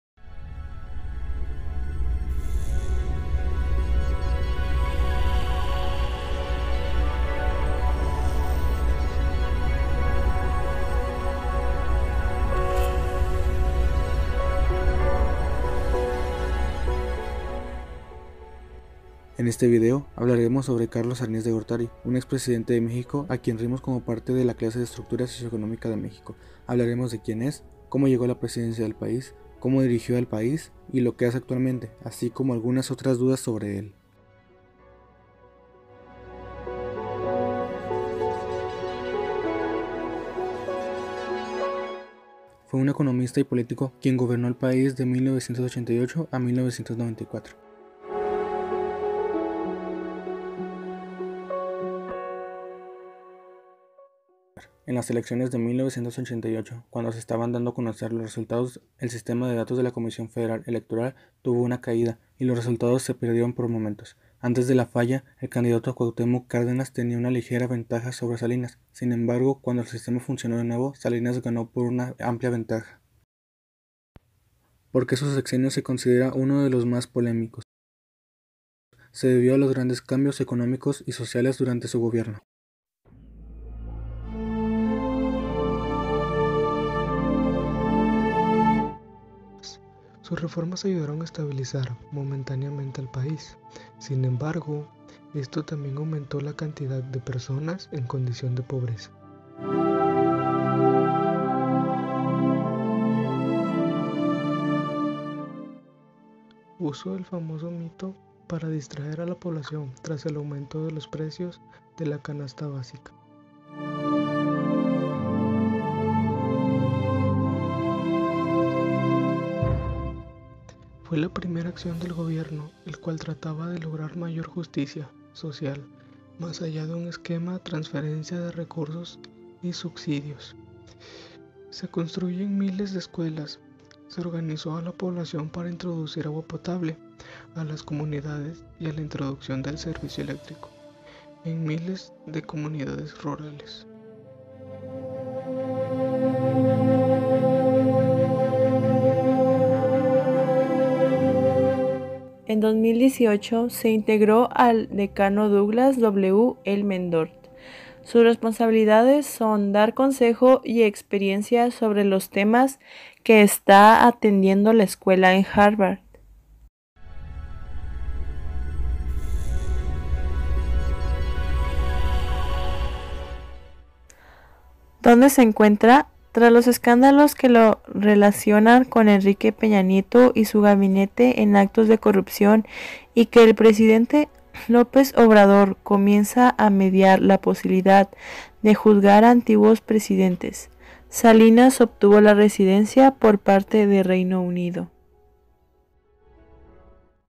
Carlos Salinas de Gortari - Estructura Socioeconómica De México
Este es un proyecto para la clase se Estructura Socioeconómica de México, elaborado por alumnos del Colegio de Bachilleres del Estado de Chihuahua Plantel No. 9. Grupo 506
Profesor: Jaime Alvarado
Alumnos:
Alemán Sánchez Juan Pablo
Arenivas Hernández Carlos
Diaz Galicia Sandra Belén
García Guadian Sergio Enrique
Quiroz Simental Vianey Priscila
Zapien Rodríguez Leslie